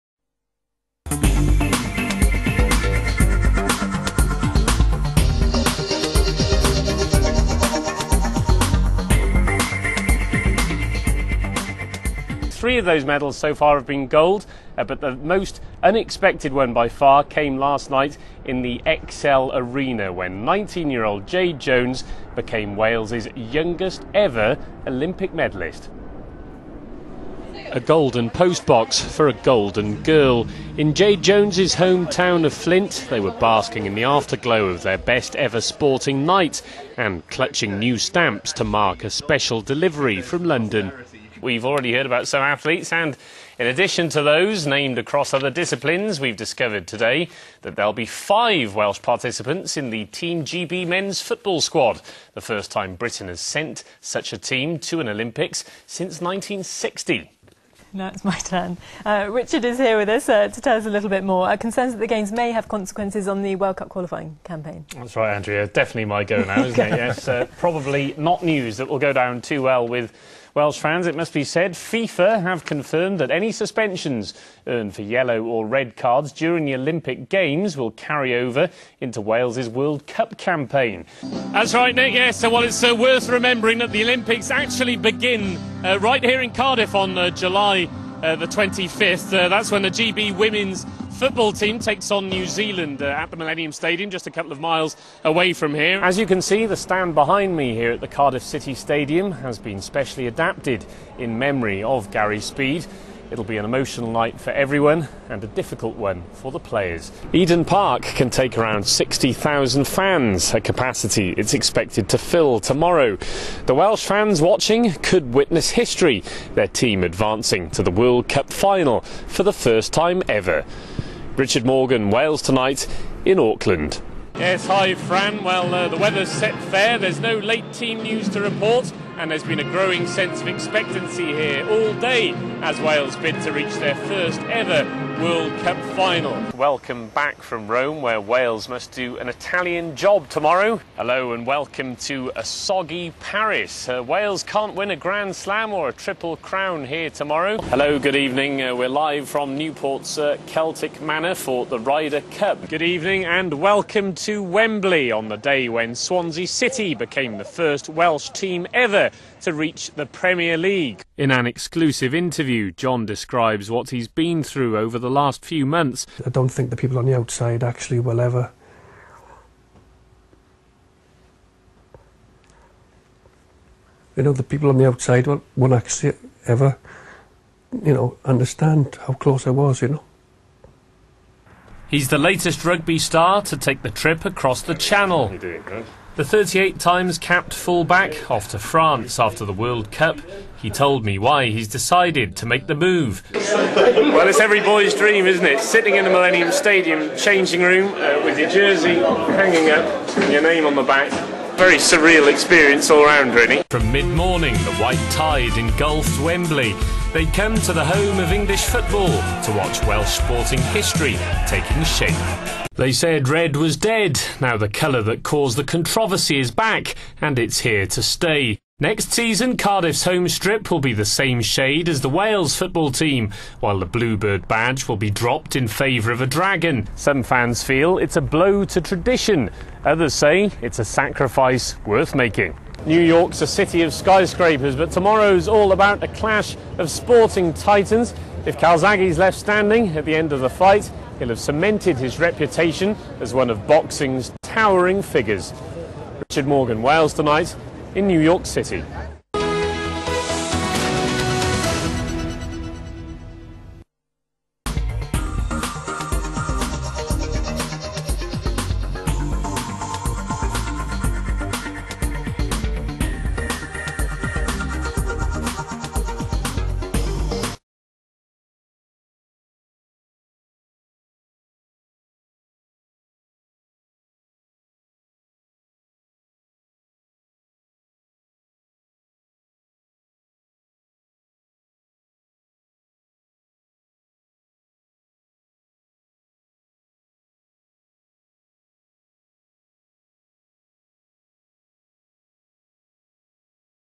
Richard Morgan sport showreel
Richard Morgan has been the sports presenter for ITV Wales since 2005. In that time he has covered three rugby Grand Slams, Swansea City's first ever season in the Premier League, the 2010 Ryder Cup at Newport's Celtic Manor, and the 2009 Ashes Test Match in Cardiff.

In 2011 he was with the Wales team in New Zealand for the Rugby World Cup, following the squad as they progressed to the semi-finals for the first time since 1987. He also covered Joe Calzaghe's 2008 'superfights' in Las Vegas and New York City.

In 2009 Richard secured an exclusive interview with Wales footballer John Hartson on his battle with cancer. His coverage won a silver award for 'scoop of the year' at the ITV News awards.

Richard is an experienced broadcaster who also regularly presents news bulletins for ITV Wales. Since 2013 he has been a News Correspondent for the channel.

In 2017, he co-authored 'The Byrne Identity', a rugby biography, with former Wales full back Lee Byrne. The book was nominated for the following year's Sports Book of the Year Awards held at Lord's Cricket ground.